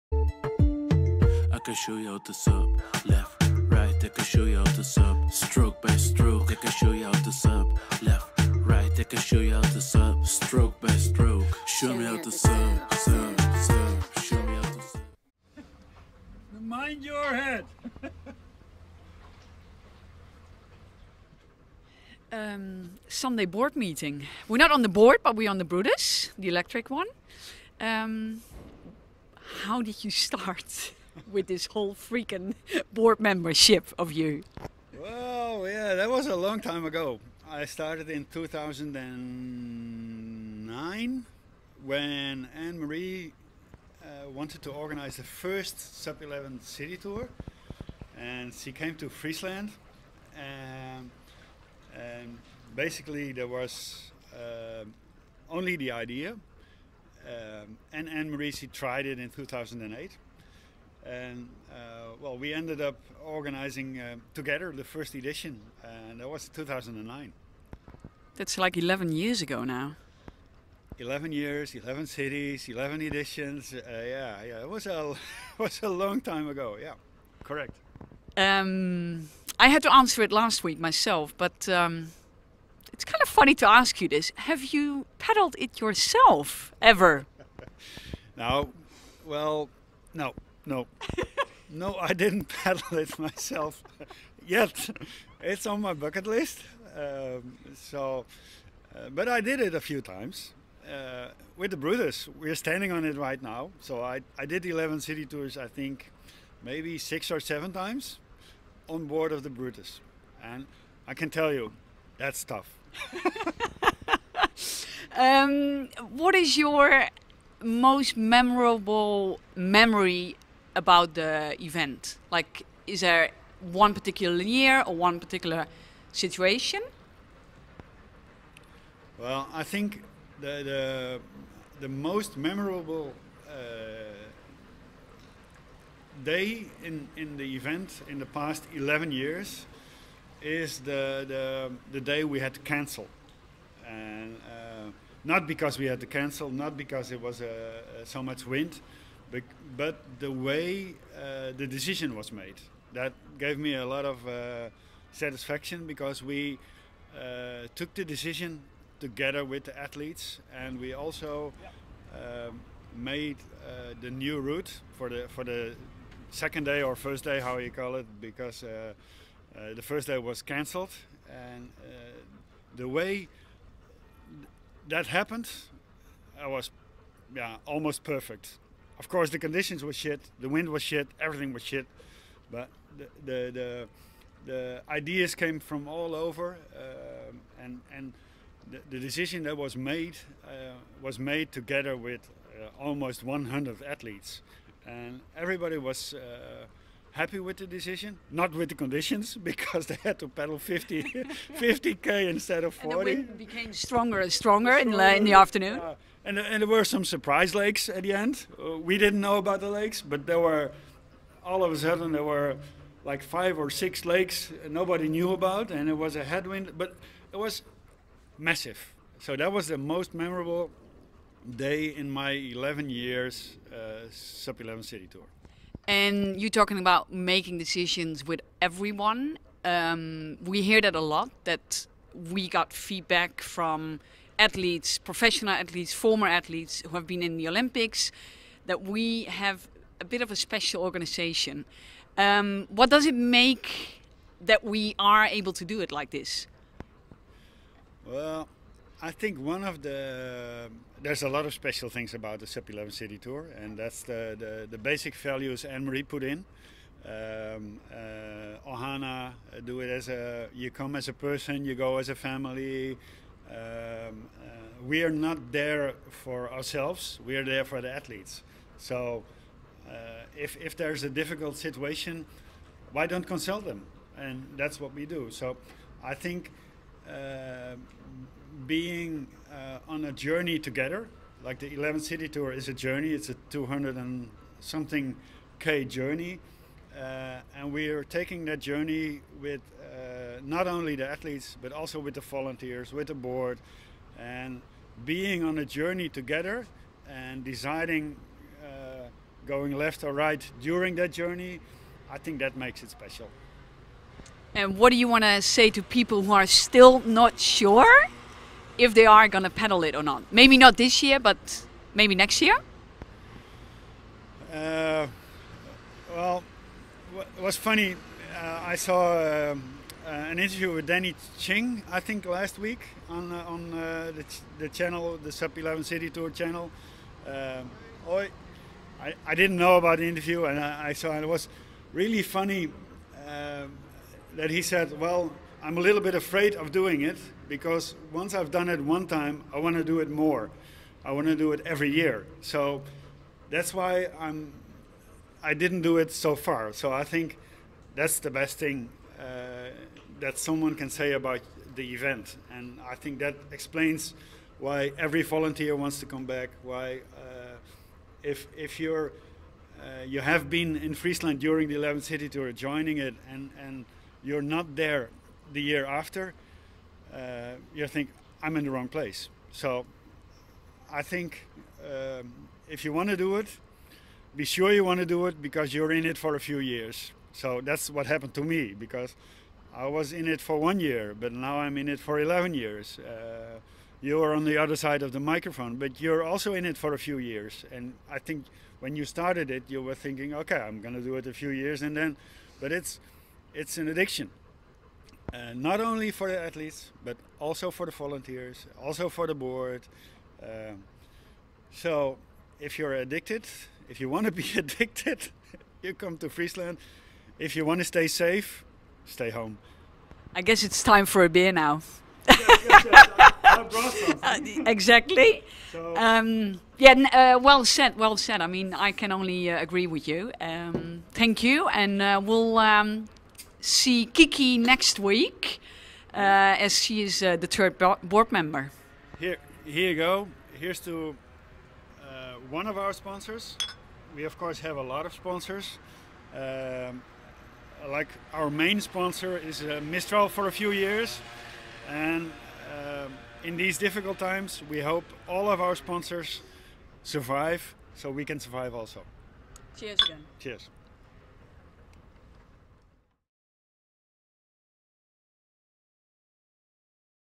SUNDAY BOARD MEETING with RITSKE MERKUS
Yes it`s sunday again, so we have another SUNDAY BOARD MEETING coming up!
Today we have chairman Ritske Merkus cruising on the Brutus through Leeuwarden with Marije while giving you some interesting insides about the SUP 11 City Tour.
Ritske became part of the SUP 11 board 11 years ago now and he knows what to do when you want to become addicted to something: Come to Friesland in SUPtember!
photo: Mayola Dijksman edit: Tanja Ecker